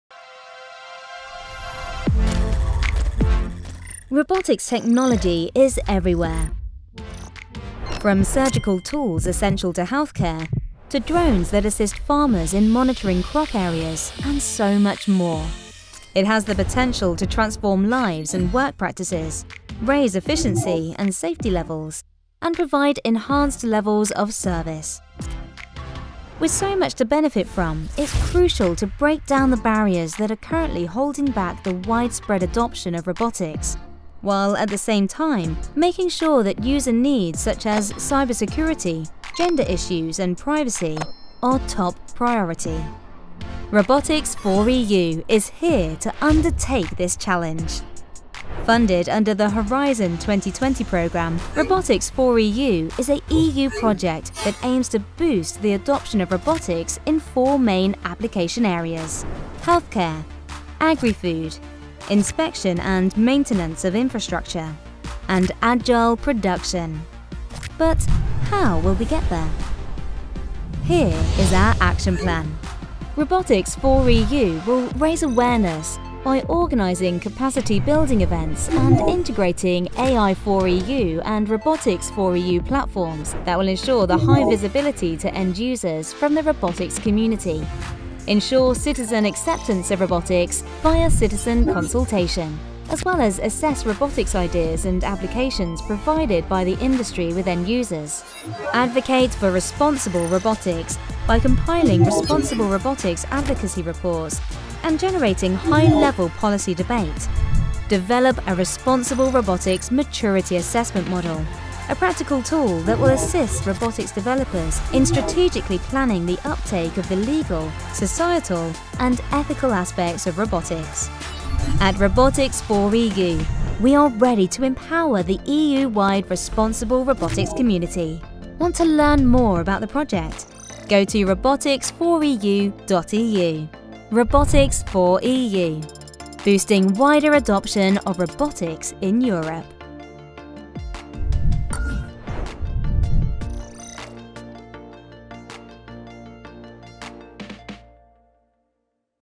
Robotics4EU - Boosting Wider Adoption of Robotics in Europe
This video introduces Robotics4EU, a three-year project, funded under the Horizon2020 programme, that will ensure a more widespread adoption of (AI-based) robots focusing on 4 application areas: Healthcare; Agri-food; Inspection and maintenance of infrastructure and Agile production.